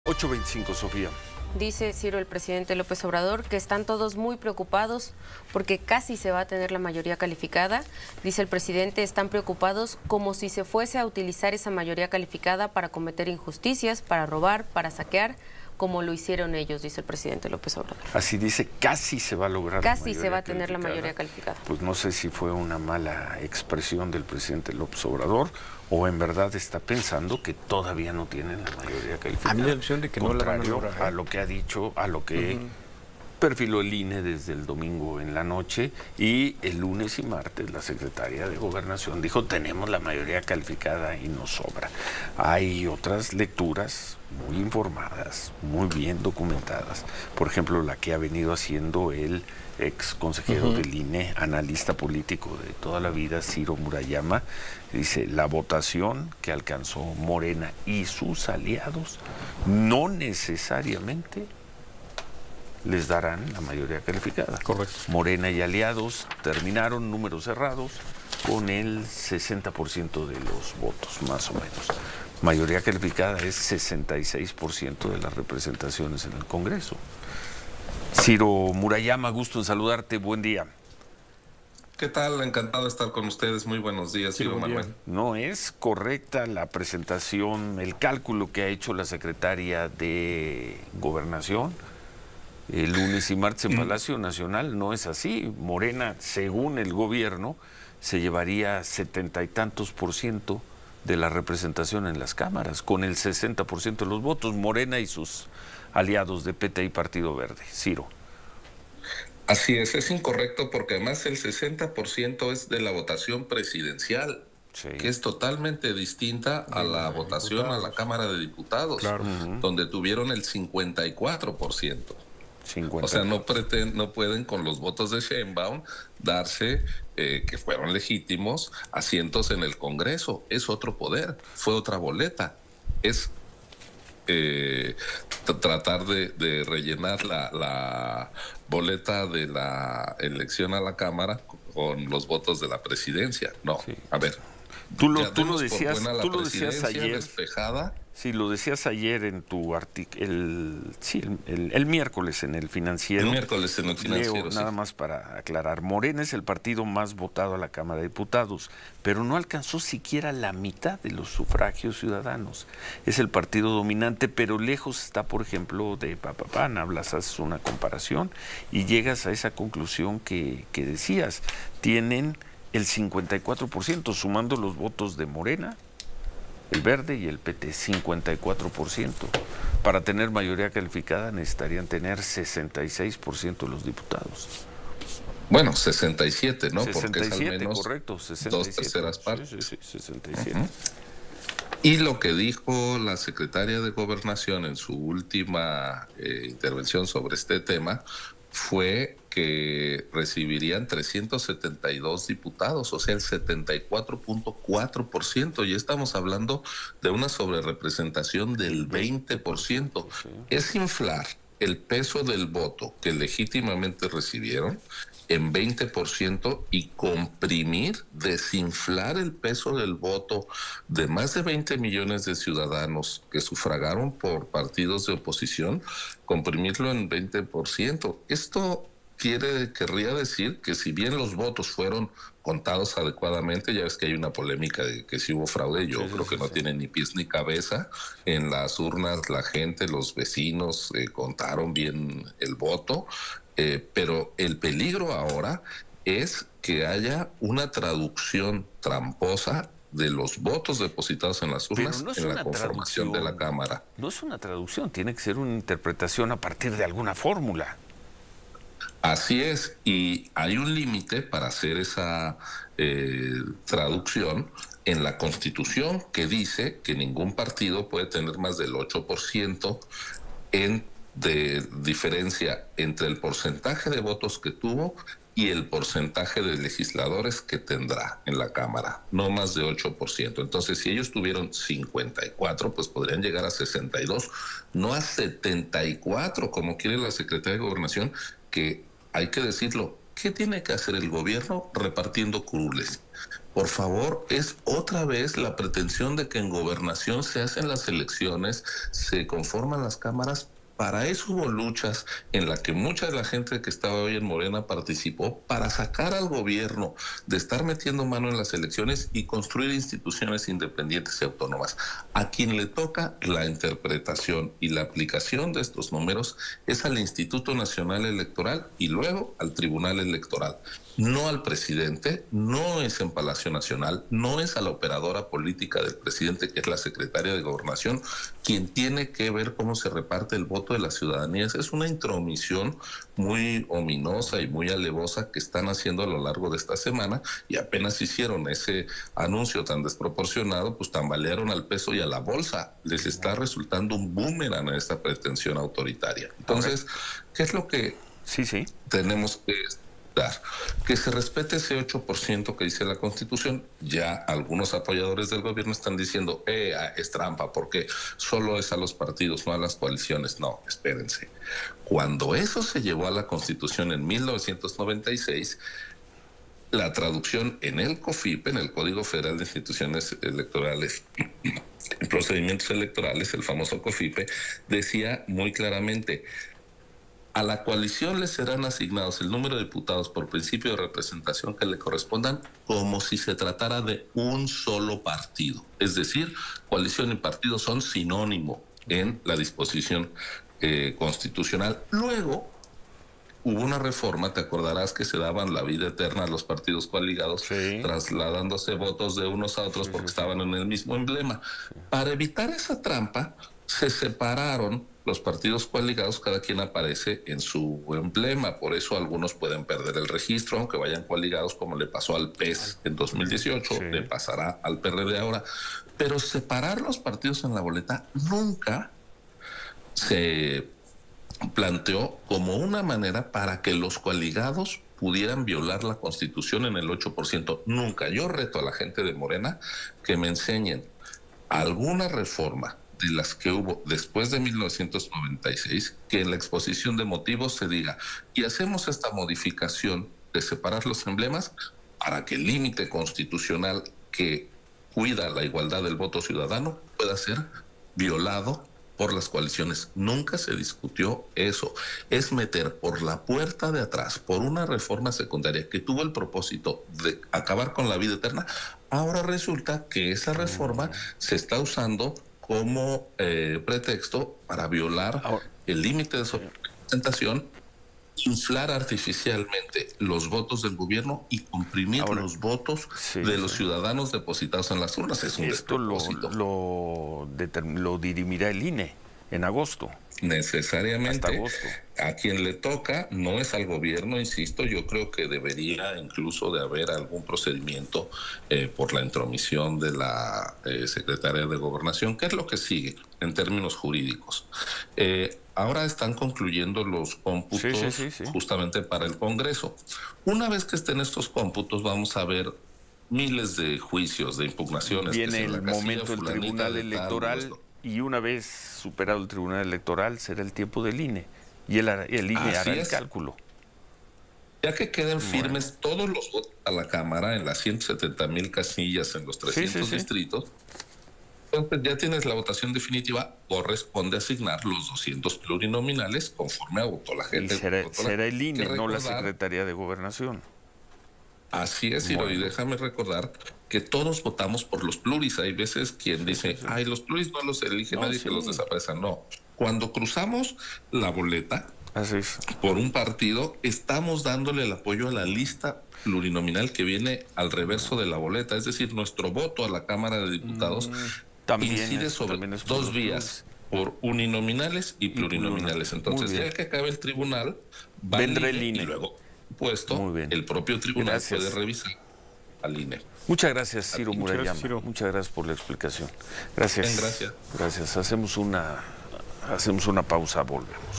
Ciro Murayama desmiente mayoría calificada de Morena en cámaras
El exconsejero del INE, Ciro Murayama, explica que Morena busca inflar la votación en la Cámara de Diputados. Asegura que no tienen la mayoría calificada ya que para tenerla requieren del 67 por ciento y sólo obtuvieron el 54 por ciento de la votación. Califica de “tramposa” la interpretación hecha por Luisa María Alcalde quien habló de esta mayoría en el Congreso.

Grupo Fórmula